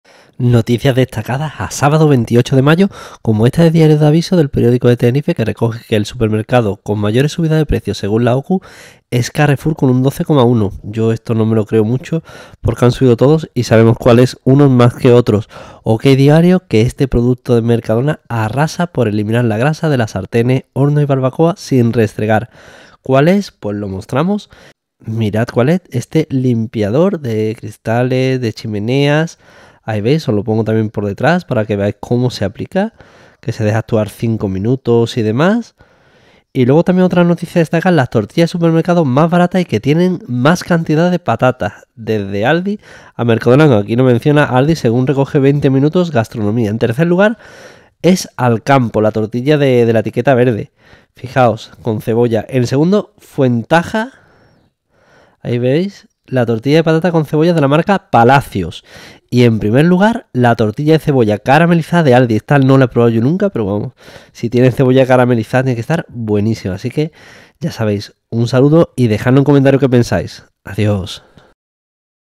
Noticias destacadas sabado 28 mayo 2022 supermercado que mas ha subido...
Noticias destacadas sabado 28 mayo 2022 supermercado que mas ha subido, limpia chimeneas, y tortilla con + patatas ¿que pensais?